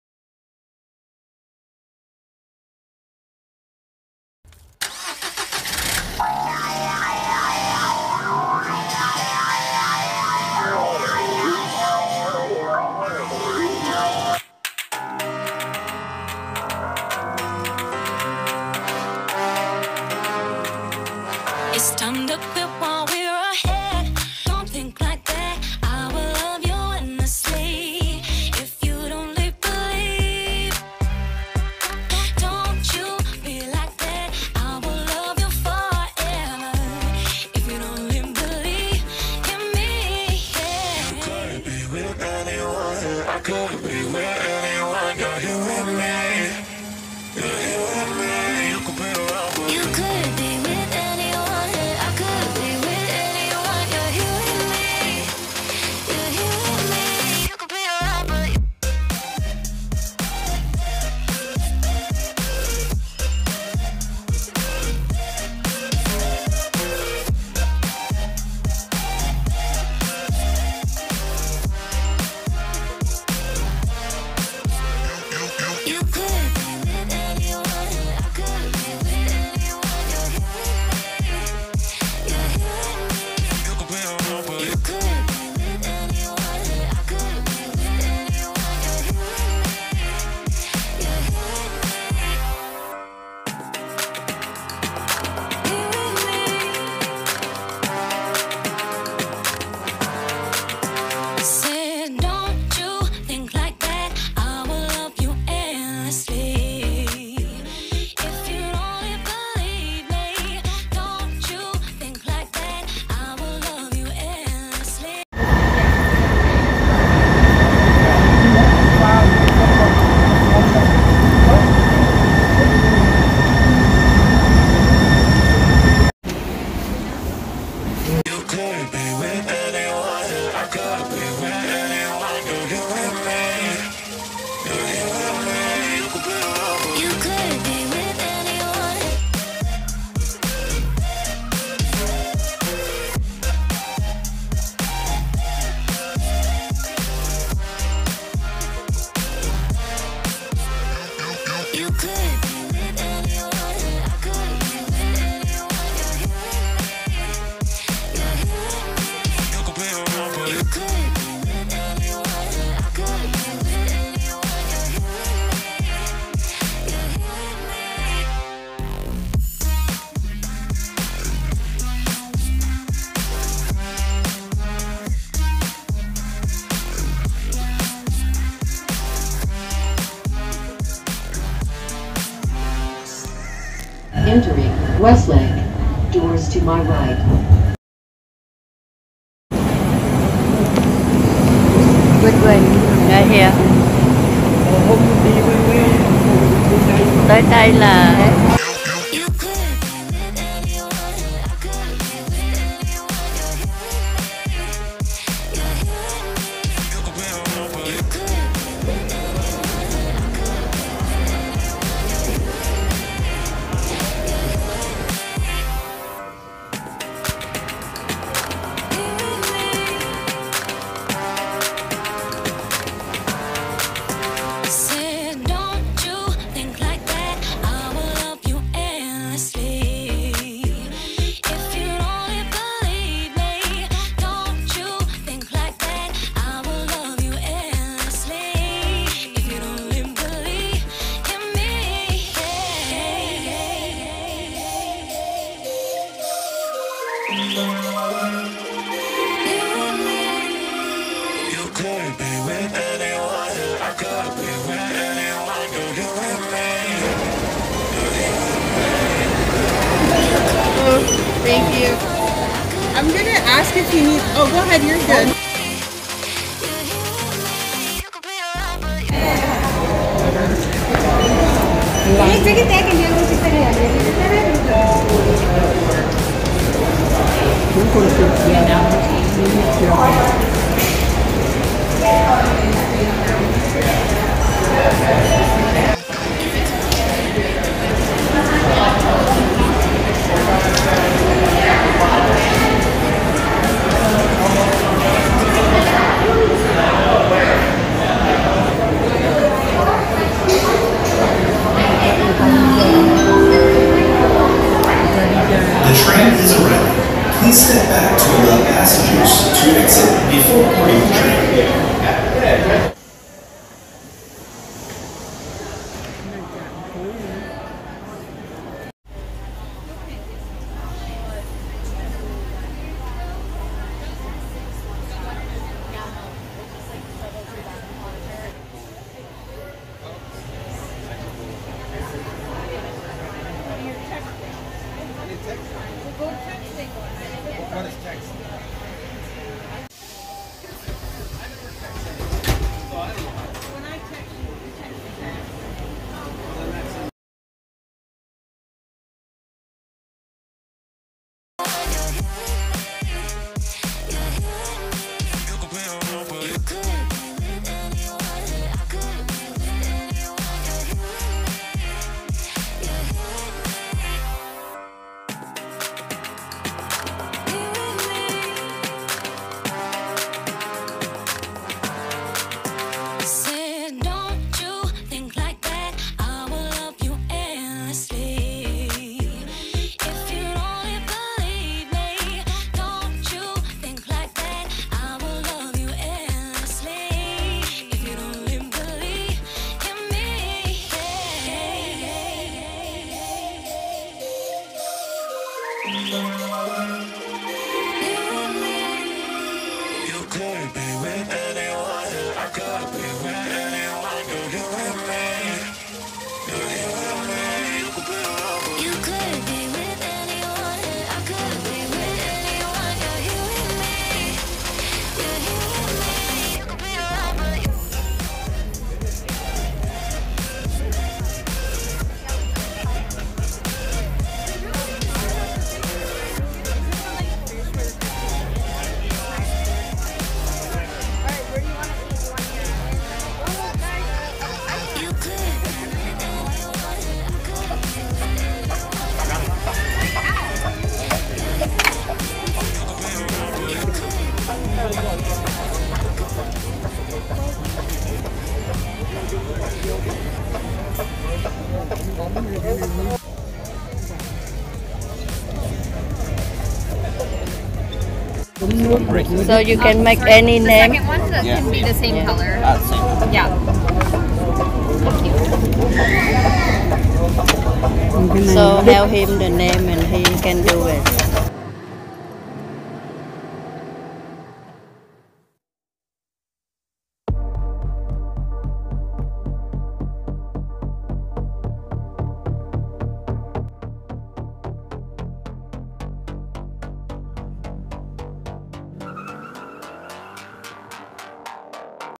#152 ĐẾN TRUNG TÂM THÀNH PHỐ SEATTLE - THÁP KIM KHÔNG GIAN.PART 1.
Chào cả nhà! Nhân dịp ngay lễ Lao động ở Mỹ được nghỉ làm nên đưa con đi chơi ờ trung tâm SEATTLE tháp có tên là :" Kim không gian”( SEATTLE CENTER SPACE NEEDLE). Chia sẻ lại kỉ niệm cùng mọi ngườ.

      ♡ Chúc cả nhà cuối tuân vui vẻ, thật nhiều nhiều sức khỏe và bình an.

               ° Chân thành cảm ơn cả nhà luôn yêu thương ủng hộ kênh mình nhé.